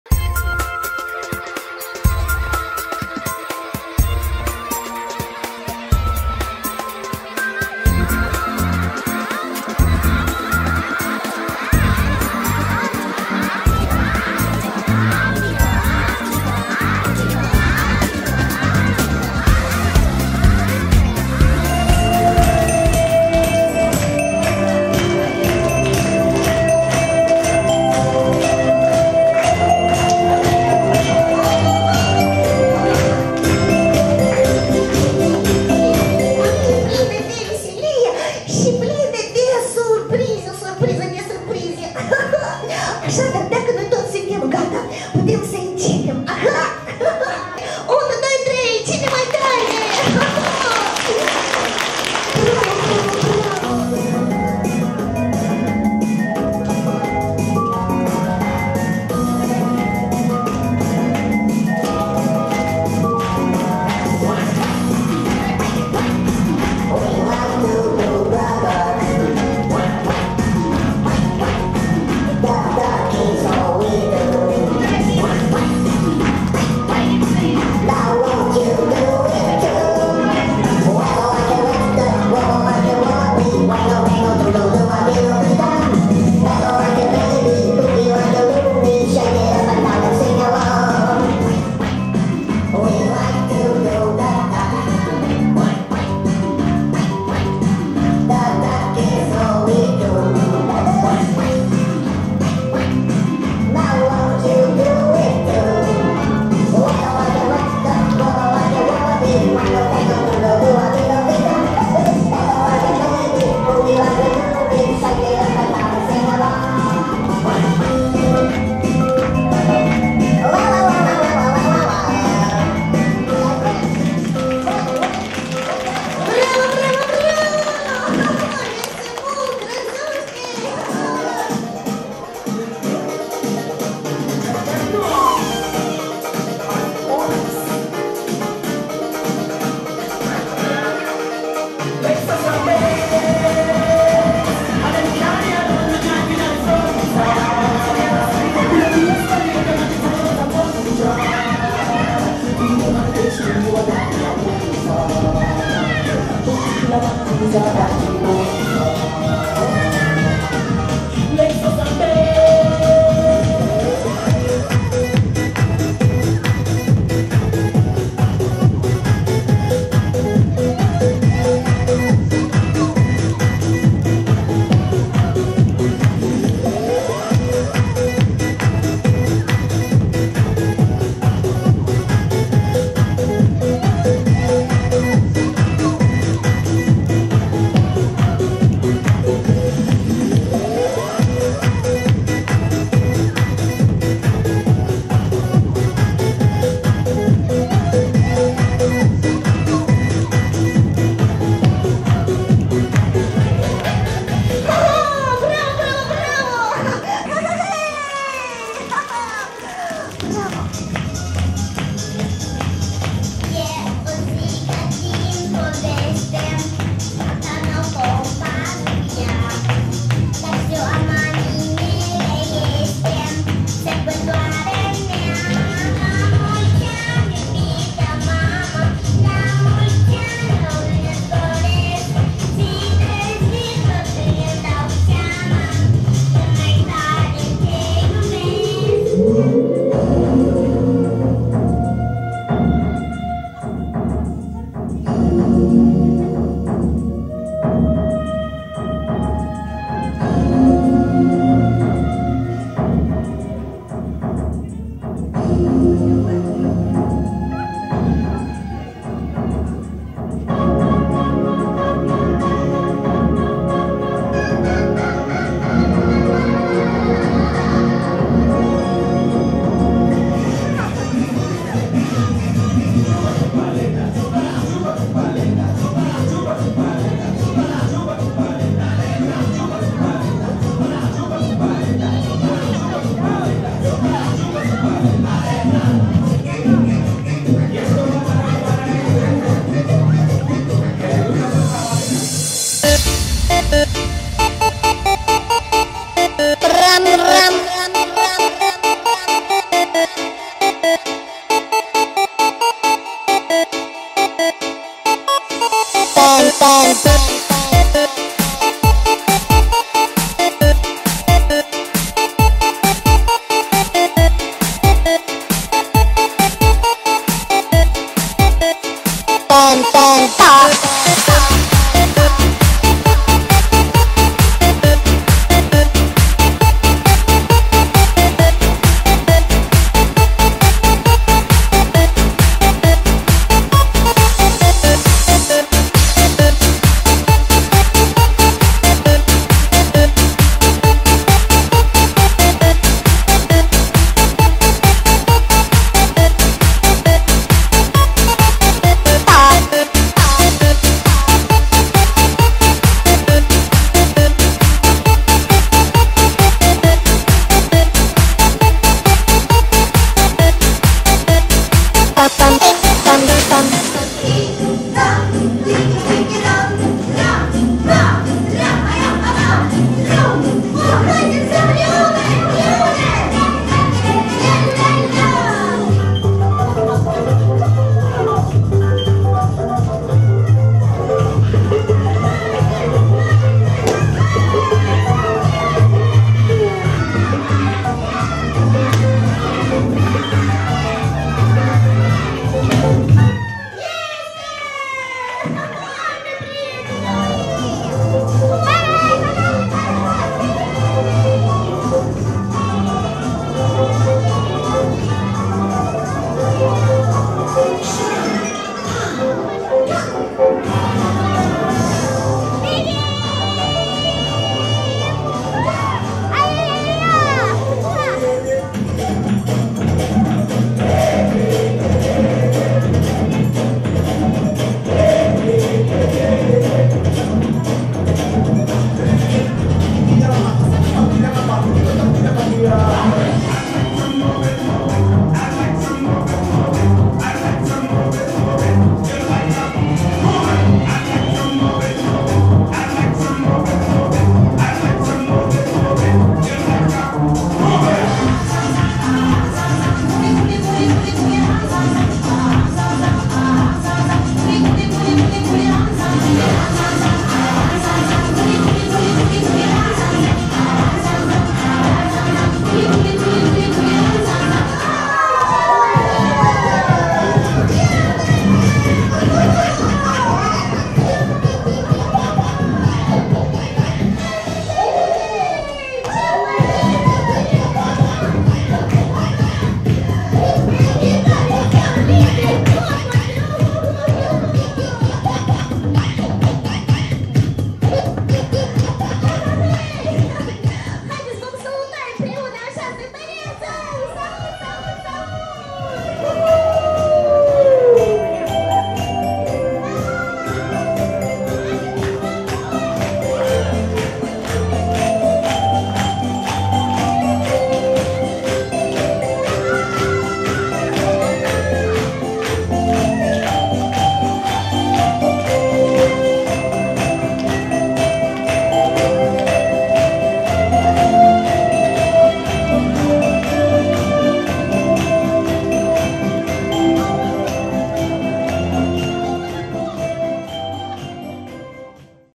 Pelerinaj Artico 2012 - Călăraşi,Ţibirica
Centrul Republican Pentru Copii si Tineret "Artico"
11 Ianuarie 2012
Artico în vizită la Călăraşi,Ţibirica - Concert cu prilejul sărbătorilor de iarnă.